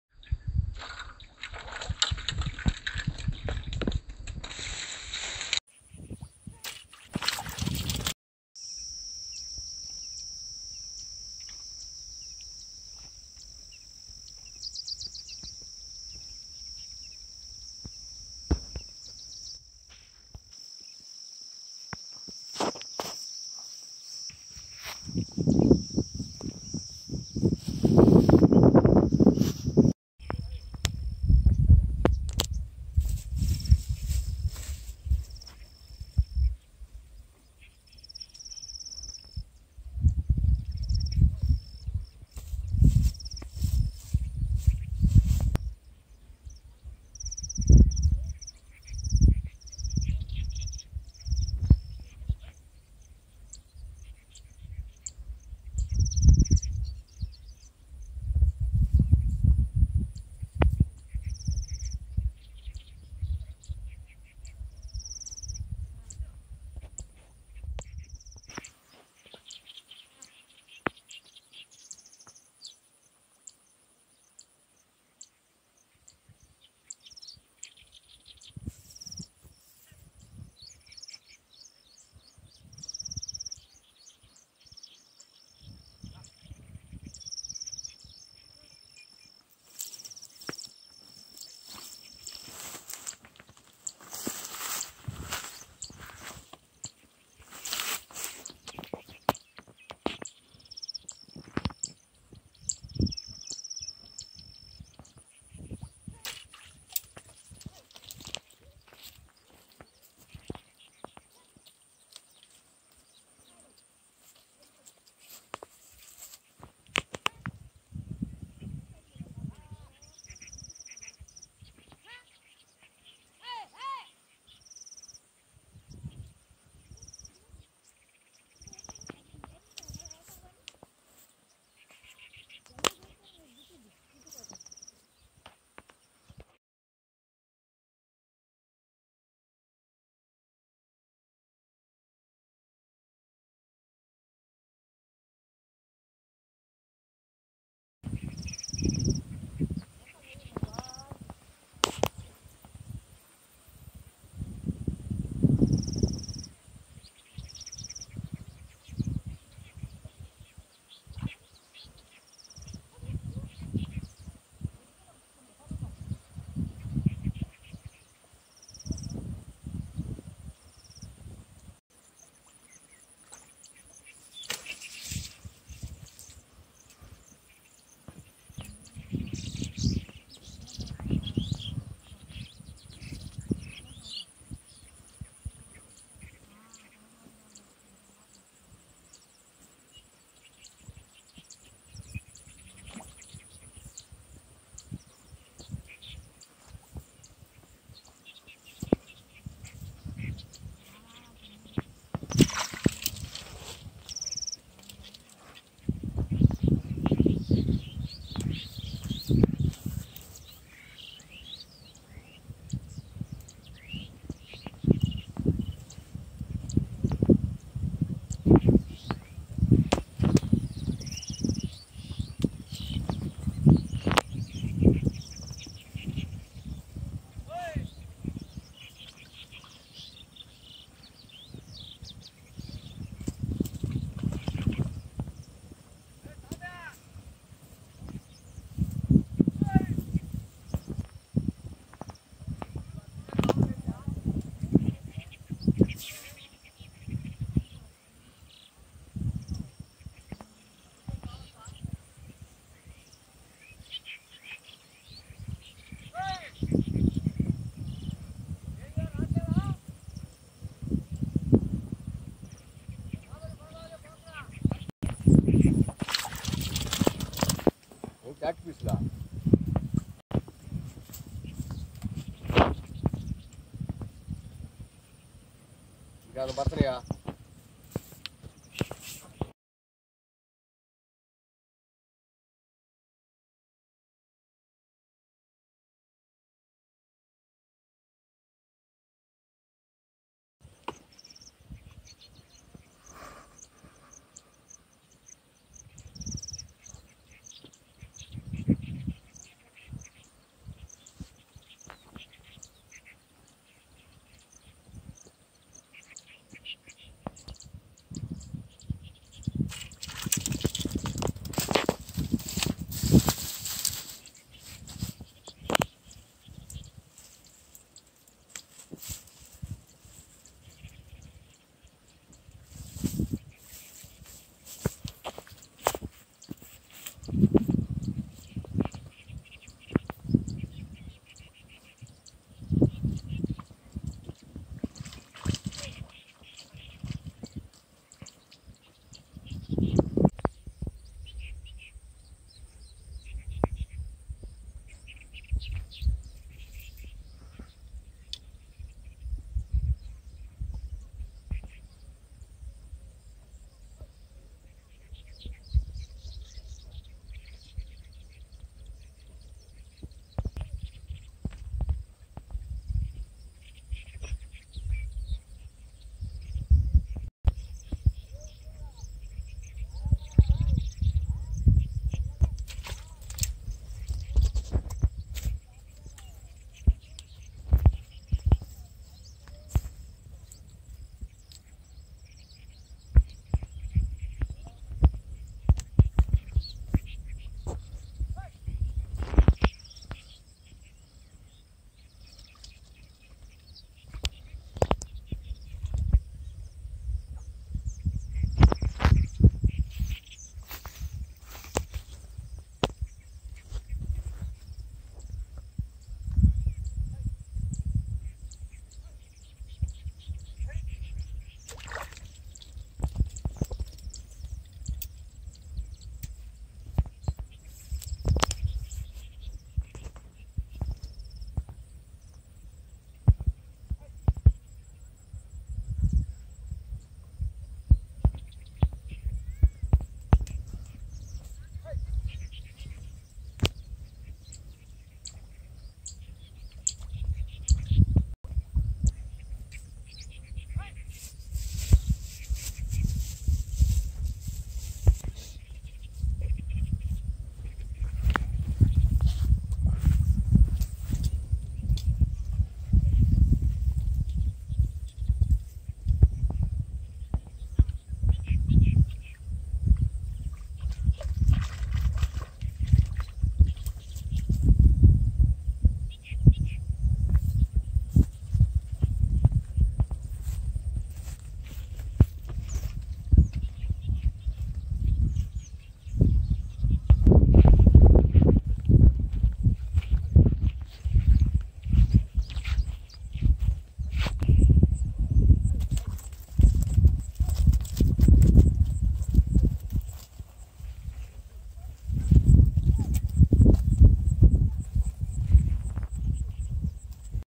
Back to back fishes catching||Catfish & Climbing perch fishes catch by hook||Amazing video
climbing perch fish and cat fish catch
we use rice bran powder to catch these fishes